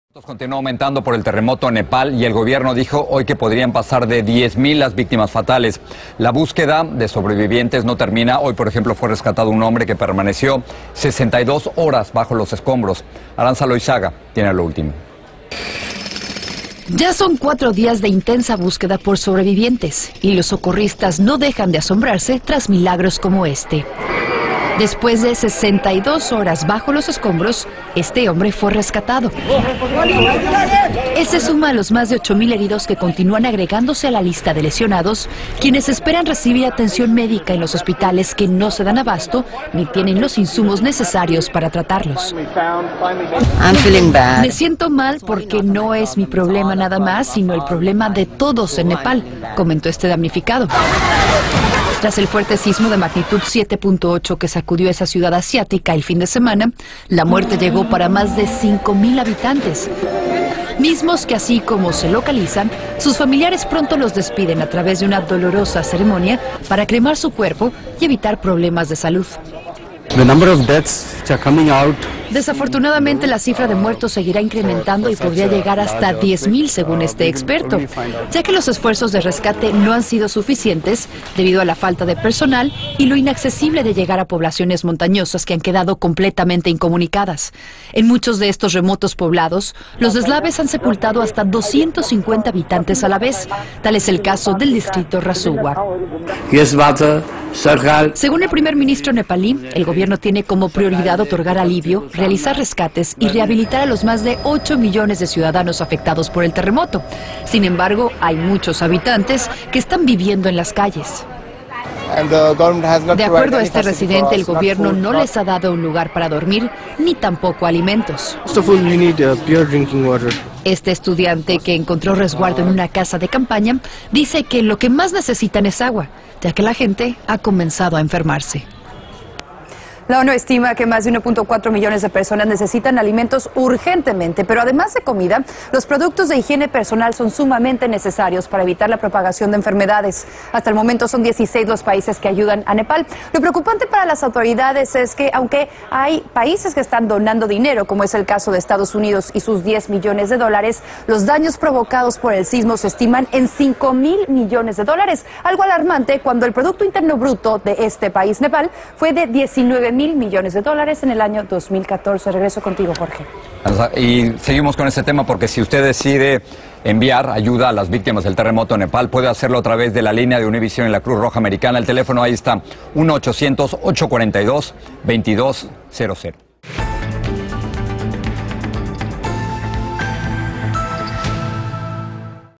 La cifra de muertos tras el terremoto en Nepal podría superar los 10 mil
Autoridades rescataron a un hombre que permaneció 80 horas bajo los escombros.